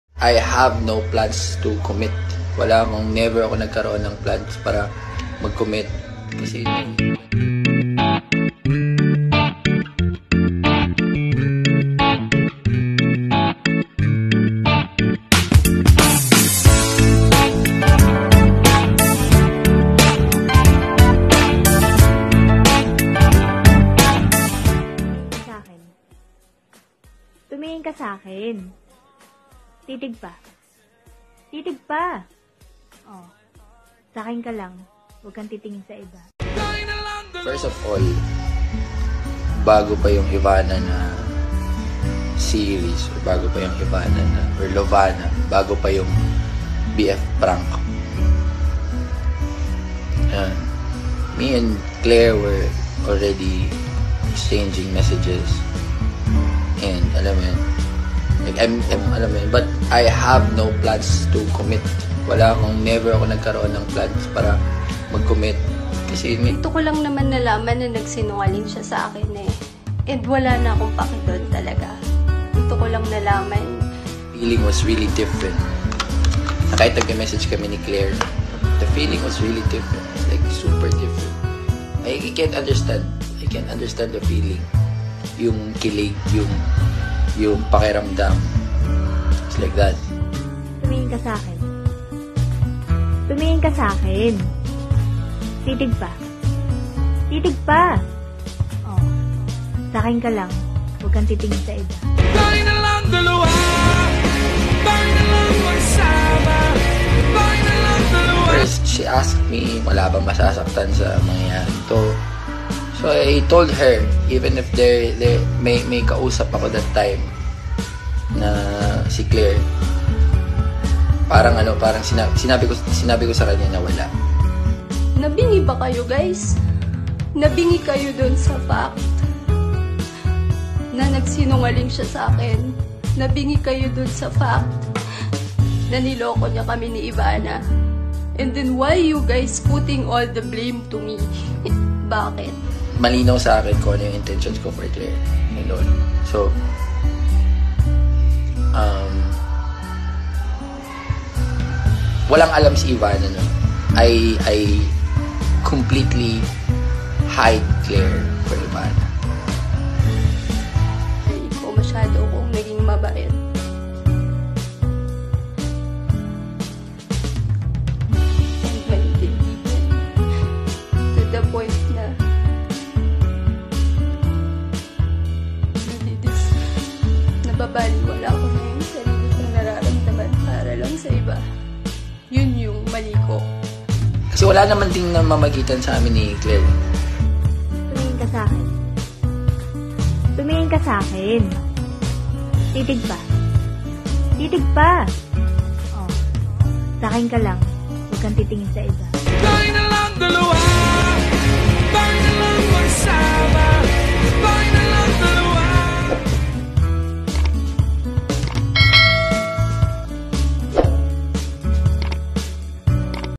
Dj Loonyo|Ivana Alawi|Claire Gomez| May sulutan bang nangyari?
Today's hot news!  Sexy actress Ivana Alawi , DJ Loonyo  and Claire Gomez. Ito ay video ng pagpapaliwanag ni DJ Loonyo sa mga issues na kumakalat na pinagsabay DAW itong sina Ivana at Claire.
Watch the video and I comment nyo ang inyong reaction.